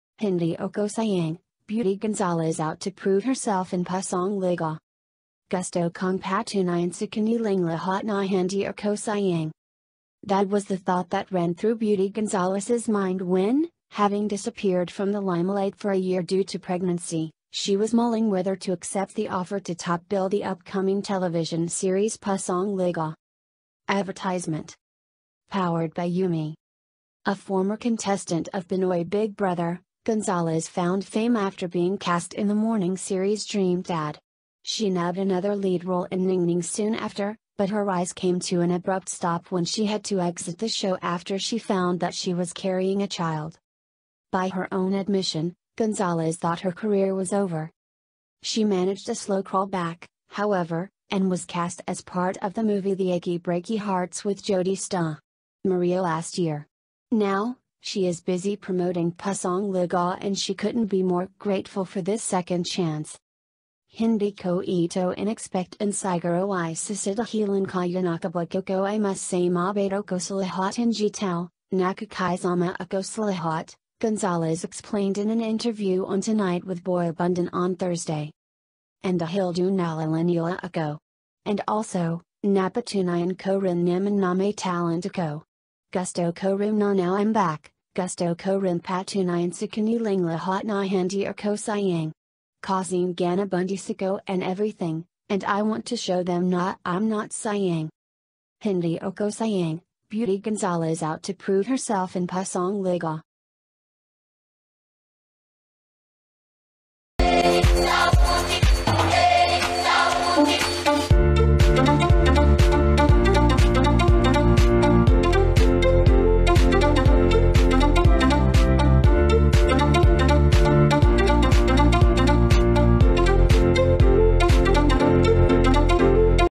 'Hindi ako sayang': Beauty Gonzalez out to prove herself in 'Pusong Ligaw'
Philippines News today : Entertaiment scandals

"Gusto kong patunayan sa kanilang lahat na hindi ako sayang."
That was the thought that ran through Beauty Gonzalez's mind when, having disappeared from the limelight for a year due to pregnancy, she was mulling whether to accept the offer to topbill the upcoming television series "Pusong Ligaw."
Advertisement

  Powered By YuMe
A former contestant of "Pinoy Big Brother," Gonzalez found fame after being cast in the morning series "Dream Dad." She nabbed another lead role in "Ningning" soon after, but her rise came to an abrupt stop when she had to exit the show after she found that she was carrying a child.
By her own admission, Gonzalez thought her career was over.
She managed a slow crawl back, however, and was cast as part of the movie "The Achy Breaky Hearts" with Jodi Sta. Maria last year. Now, she is busy promoting "Pusong Ligaw" --and she couldn't be more grateful for this second chance.
"Hindi ko ito in-expect and siguro isa sa dahilan kaya nakabalik ako --I must say mabait ako sa lahat ng tao, nakikisama ako sa lahat," Gonzalez explained in an interview on "Tonight with Boy Abunda" on Thursday.
"And dahil doon naalala nila ako. And also, napatunayan ko rin naman na may talent ako. Gusto ko rin na now I'm back, gusto ko rin patunayan sa kanilang lahat na hindi ako sayang. Kasi nga nabuntis ako and everything, and I want to show them na I'm not sayang."